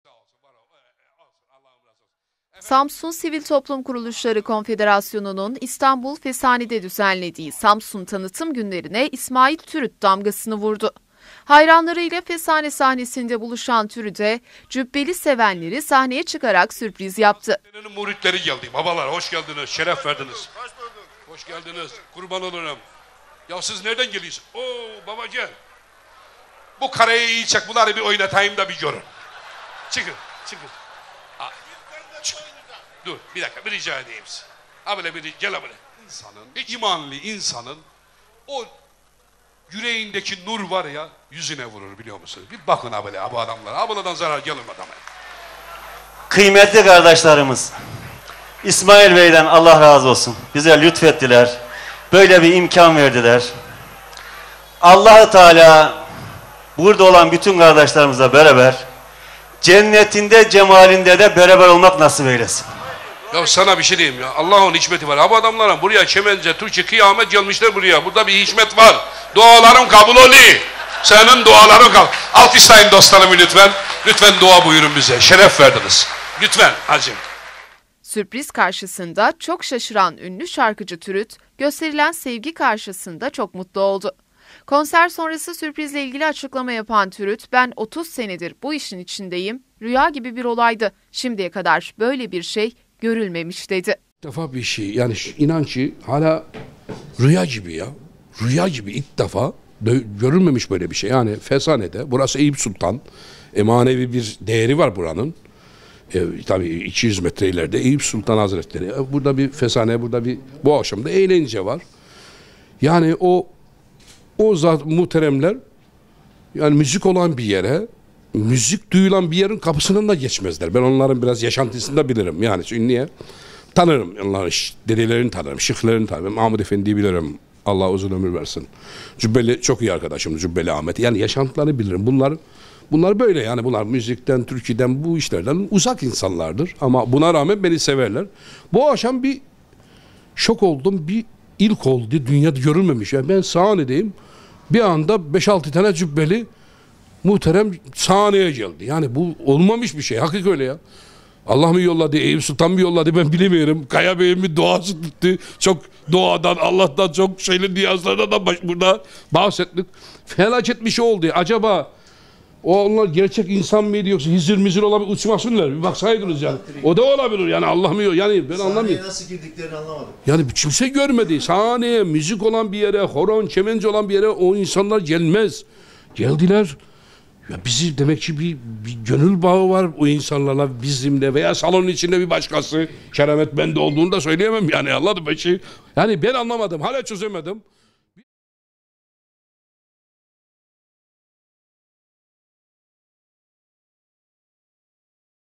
CÜBBELİLER İSMAİL TÜRÜT'ÜN SAHNESİNİ BASTI
Samsun Sivil Toplum Kuruluşları Konfederasyonu'nun düzenlediği Samsun Tanıtım Günleri Fuarı'nda sahne alan ünlü Karadenizli şarkıcı İsmail Türüt'ün cübbeli hayranları sahneyi bastı. Haberlerin Devamı